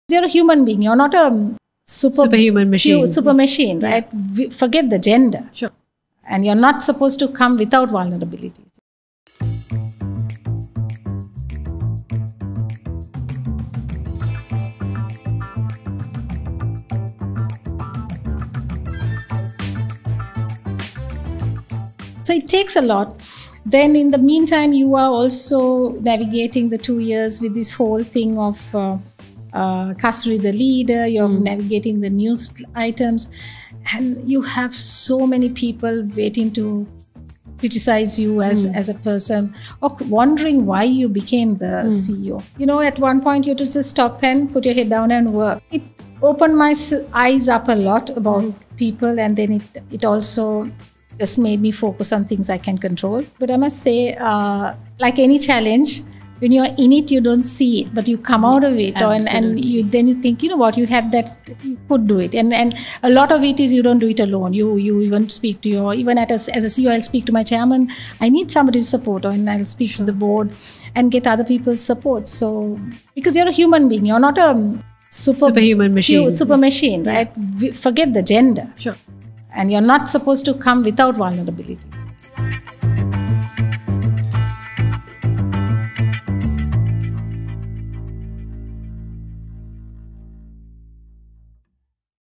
Kasturi Chellaraja Wilson on being vulnerable | She Slays 2022 Highlights
Powering through the crisis highlights | She Slays 2022

Kasturi Chellaraja Wilson was appointed the CEO of Hemas Holdings during the pandemic. In an interview with Randhula de Silva, she discusses powering through the crisis, being a role model, and her vision for Hemas.